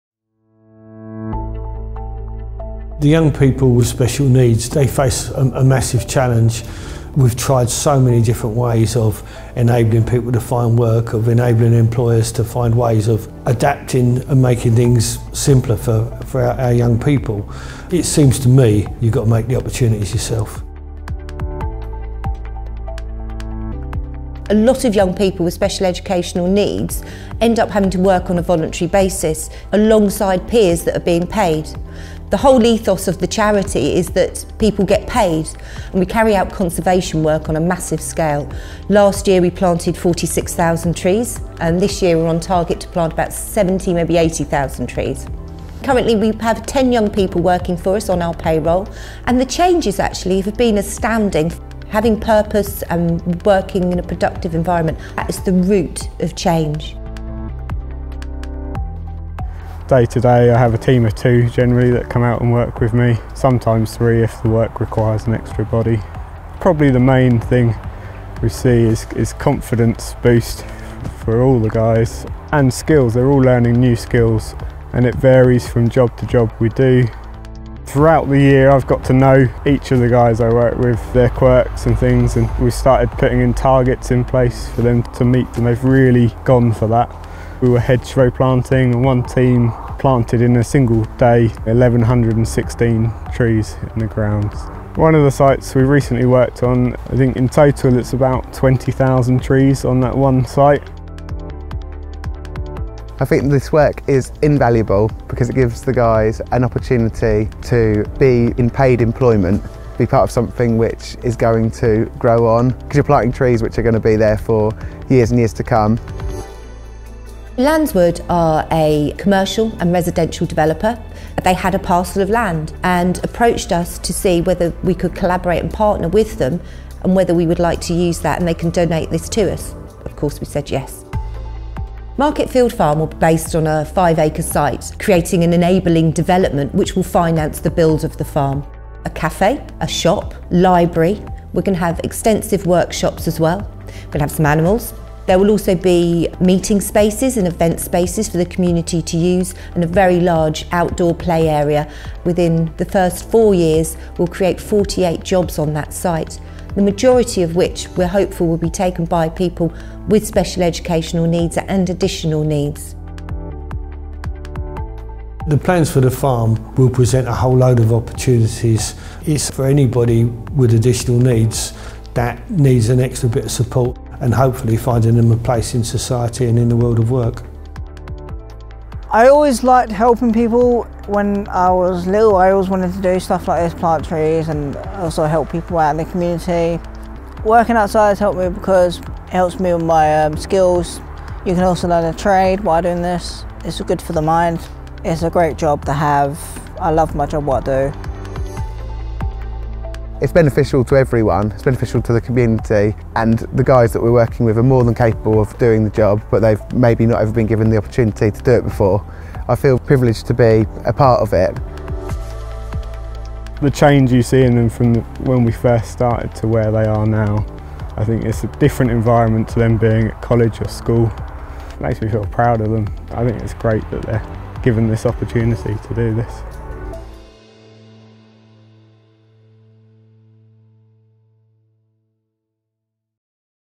Market Field Farm
Market Field Farm enables young adults with a learning disability, autism and/or ADHD to access meaningful paid employment. Working with local people, companies and parish councils to conserve natural habitats, create wildlife-friendly gardens and help maintain public spaces with conservation and nature in mind, the charity gives young people with learning disabilities a stepping stone into work.
We first met Market Field Farm when they won one of our Grants for Good in 2021. Since then they have been going from strength to strength as they expand, and for 2023 we've funded this film for them - created by the wonderful  @bigpicturecharityfilms8497